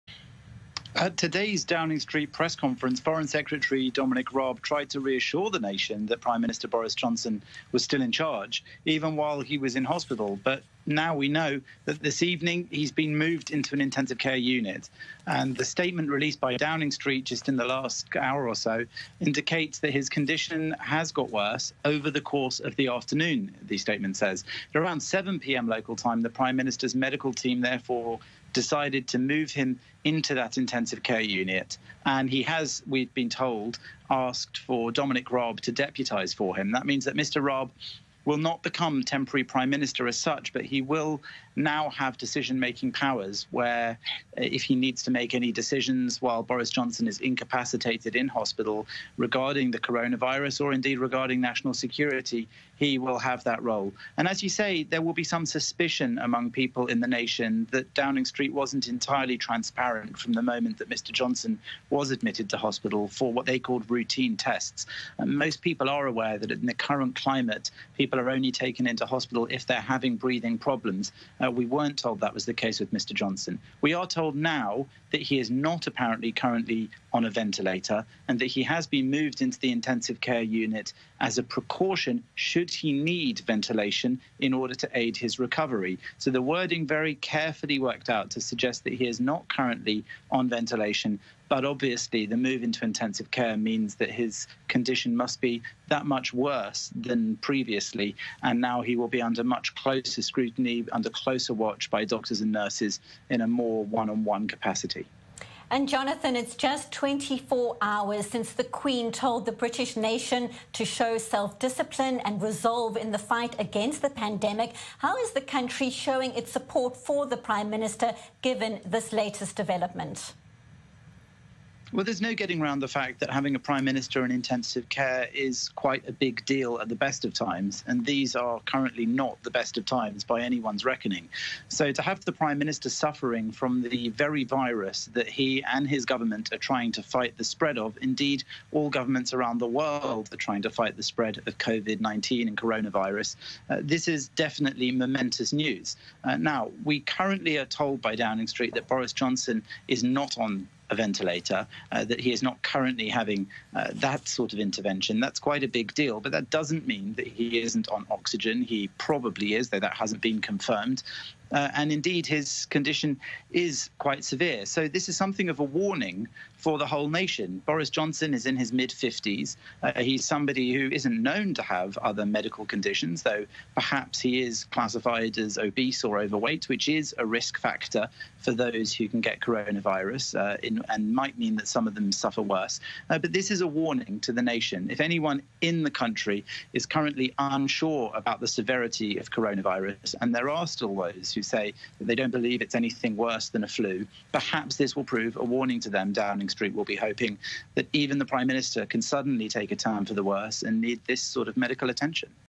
Boris Johnson taken into intensive care
i24news UK and Europe correspondent Jonathan Sacerdoti reports.